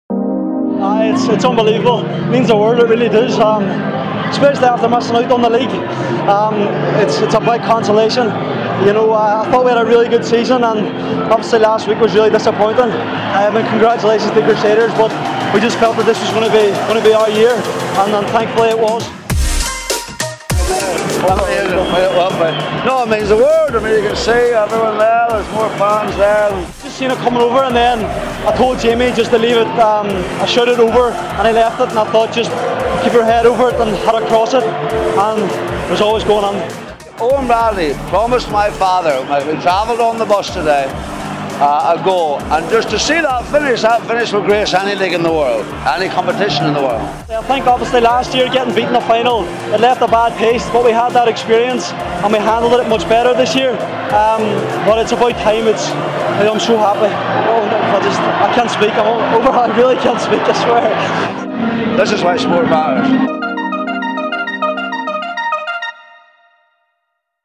Darren McCauley on Coleraine's Tennent's Irish Cup Final victory
We hear from goalscorer Darren McCauley and lifelong Coleraine fan Jimmy Nesbitt after their 3-1 win in the Tennent's Irish Cup Final.